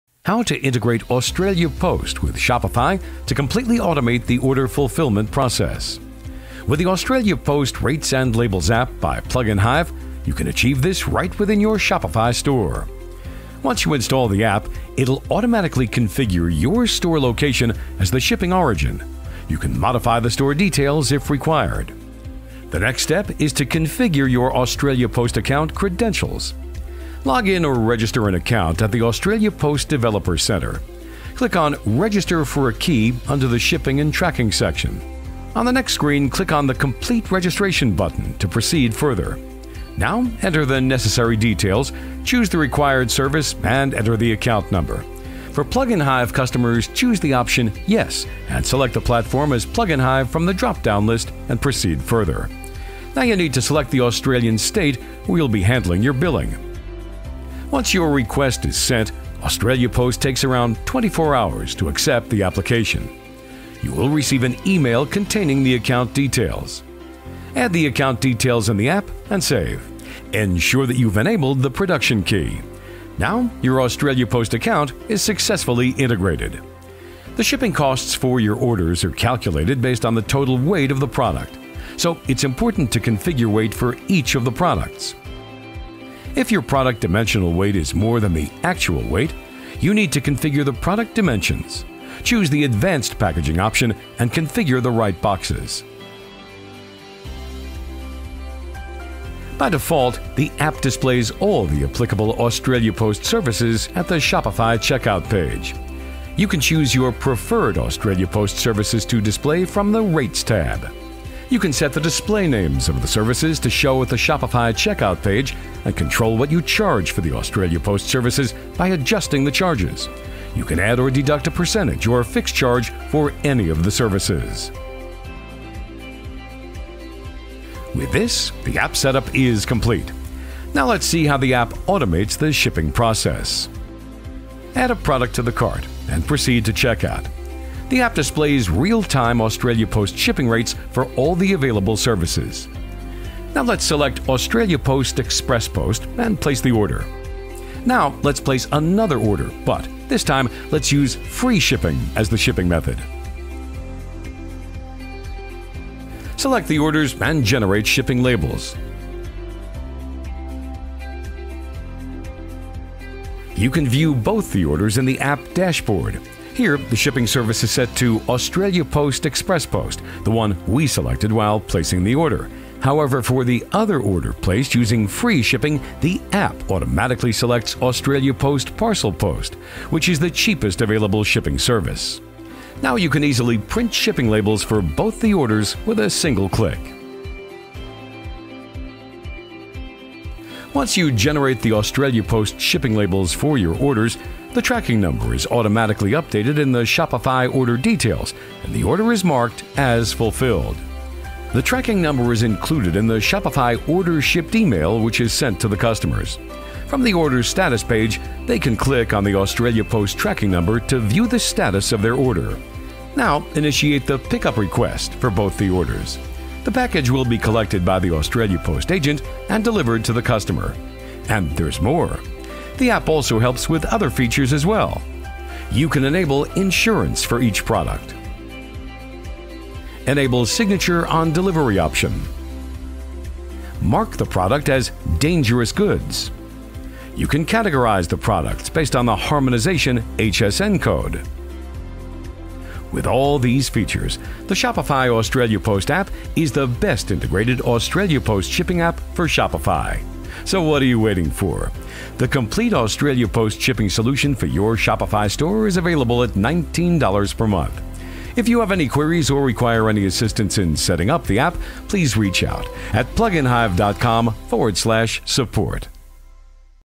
How to integrate Australia Post with Shopify to completely automate the order fulfilment process?
With the Australia Post Rates & Labels App by PluginHive, you can achieve this right within your Shopify store.

Once you install the App, it will automatically configure your Store Location as the Shipping Origin. You can modify the Store details if required.

The next step is to configure your Australia Post account credentials.

- Login or Register an account at the Australia Post Developers Centre
- Click on “Register for a key” under the shipping and tracking section
- On the next screen, click on the “Complete Registration” button to proceed further.
- Now, enter the necessary details, choose the required service and enter the account number. A general API key will be generated for the selected services.
- For PluginHive customers, choose the option “Yes” and select the platform as PluginHive from the dropdown list and proceed further.
- Now, you need to select the Australian state where you will be handling your billing.
- Once your request is sent, Australia Post takes around 24 hours to accept the application. You will receive an email containing the account details.

Add the account details in the app and save. Ensure that you have enabled the Production Key. Now, your Australia Post account is successfully integrated.

The shipping costs for your orders are calculated based on the total weight of the product. So, it is important to configure weight for each of the products. If your product dimensional weight is more than the actual weight, you need to configure the product dimensions, choose the advanced packaging option and configure the right boxes.

By default, the app displays all the applicable Australia Post services at the Shopify checkout page. You can choose your preferred Australia Post services to display from the Rates tab.

You can set the display names of the services to show at the Shopify checkout page and control what you charge for the Australia Post services by adjusting the charges. You can add or deduct a percentage or fixed charge for any of the services.

With this, the app set up is complete. Now, Let’s see how the app automates the shipping process.

Add a product to the cart, and proceed to checkout. The app displays real-time Australia Post shipping rates for all the available services.

Now, Let us select Australia Post Express Post, and place the order.

Select the orders and generate shipping labels. You can view both the orders in the app dashboard. Here, the shipping service is set to Australia Post Express Post, the one we selected while placing the order.

Now, you can easily print shipping labels for both the orders with a single click.

Once you generate the Australia Post shipping labels for your orders, the tracking number is automatically updated in the Shopify Order details and the order is marked as fulfilled.

The tracking number is included in the Shopify “Order shipped” email, which is sent to the customers. From the Order Status page, they can click on the Australia Post tracking number to view the status of their order.

Now, initiate the pickup request for both the orders. The package will be collected by the Australia Post agent and delivered to the customer.

And there’s more. The app also helps with other features as well.

You can enable insurance for each product.
Enable Signature on delivery option.
Mark the product as Dangerous goods
You can categorise the products based on the Harmonization (HSN) code
With all these features, the Shopify Australia Post App is the best integrated Australia Post shipping app for Shopify.! So what are you waiting for? The complete Australia Post shipping solution for your Shopify store is available at $19/month.